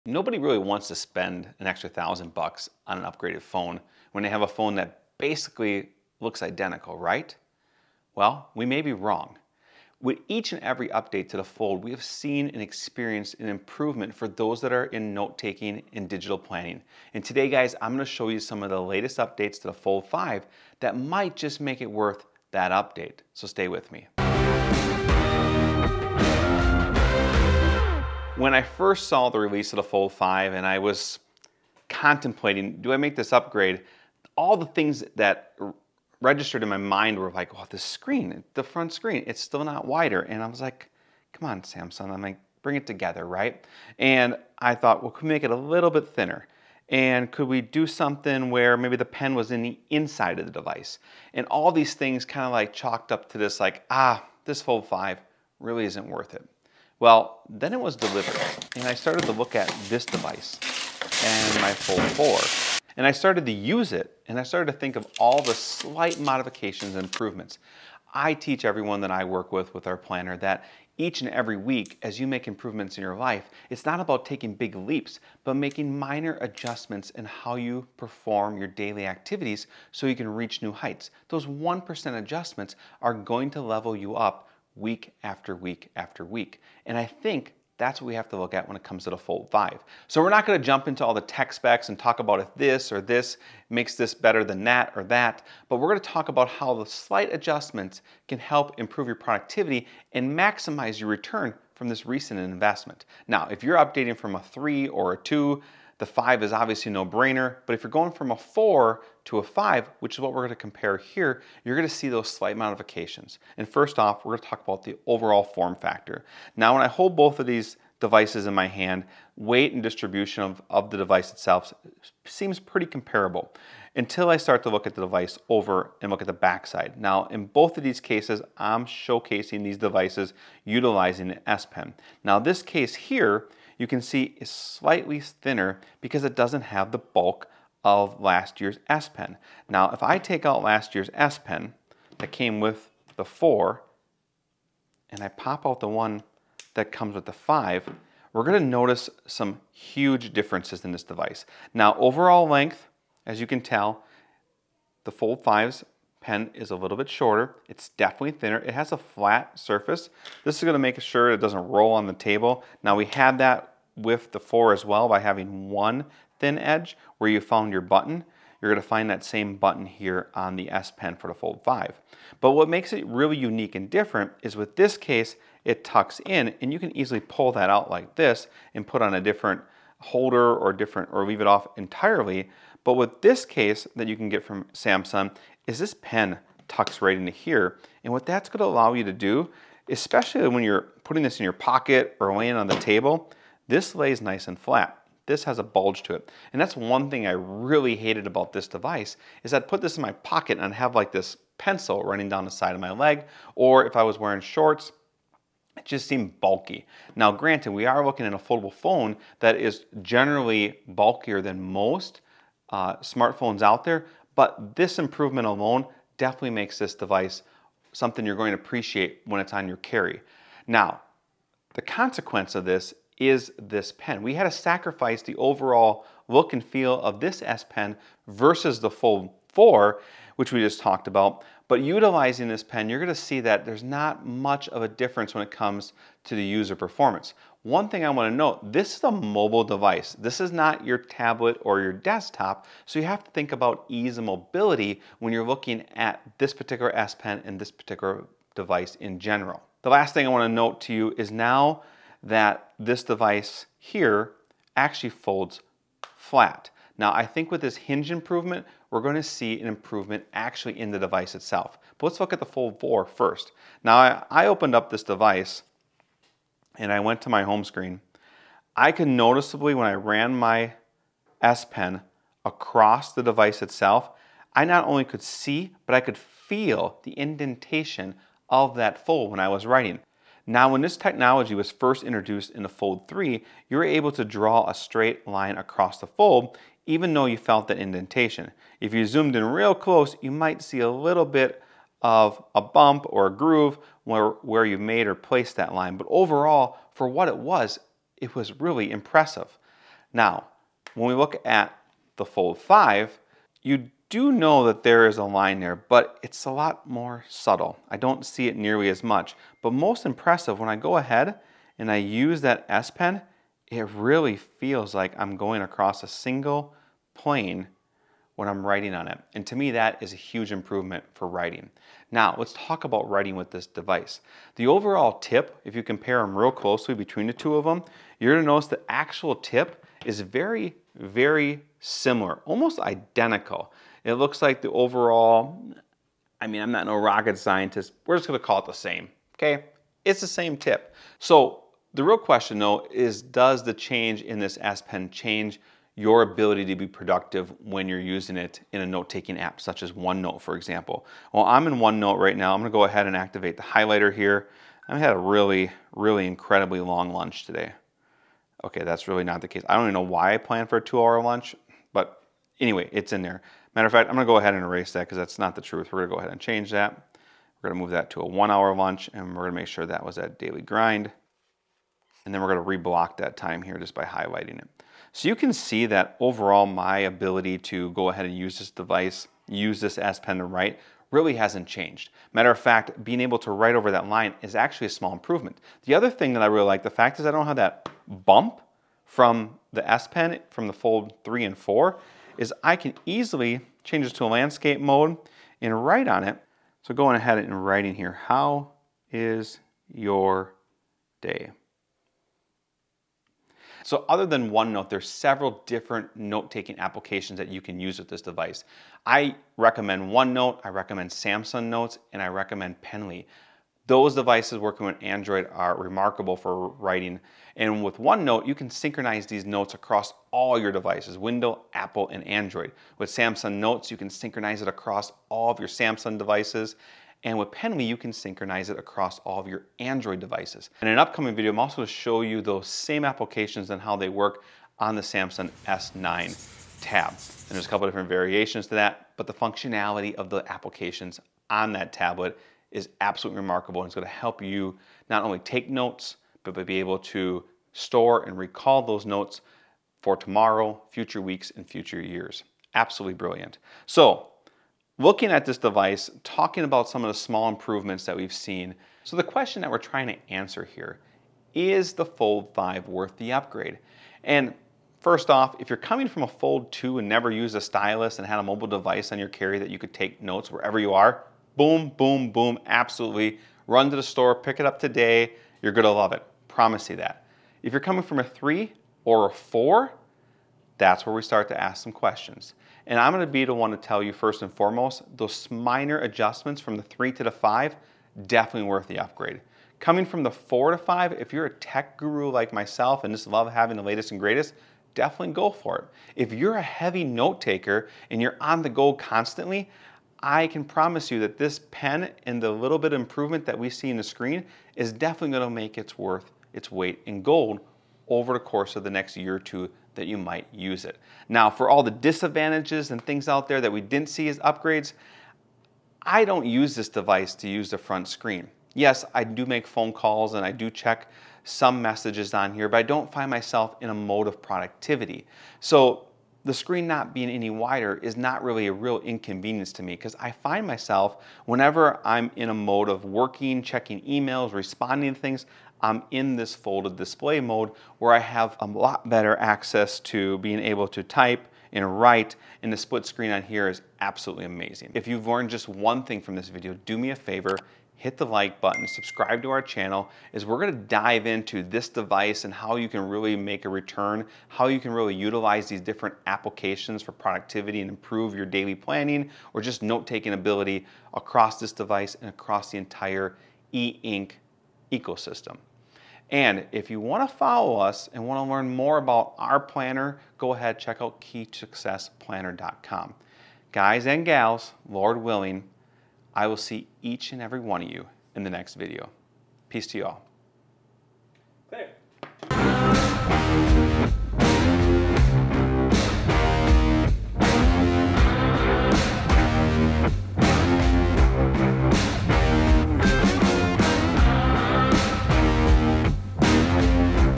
3 Reasons Why Upgrading to the Fold 5 is Right for Digital Note-taking | S Pen Review
Exploring the Upgrades: Is the Samsung Galaxy Fold 5 Worth the Investment?

Are you considering upgrading to the latest Samsung Galaxy Fold 5, but wondering if the improvements justify the investment? In this blog post, we'll delve into the key updates of the Fold 5 and discuss how they can enhance your productivity and digital planning experience. (Looking for a digital planner for your Samsung?

0:00 Intro
0:30 The upgrades we were expecting
1:40 Z Fold 5 vs Z Fold 4
2:04 New Z Fold 5 S Pen vs Z Fold 4
4:00 New Hinge in Fold 5 vs Fold 4
5:10 Using S Pen on the Z Fold 5
5:50 Using OneNote on the Z Fold 5
6:44 Which Note-Taking Apps work with Z Fold 5
7:43 Is it worth upgrading to the Z Fold 5
9:10 Digital Planner for Z Fold 5

Please consider following me for life, business, creative and communication hacks at any of the following:

We look forward to the next year and reaching our goals together.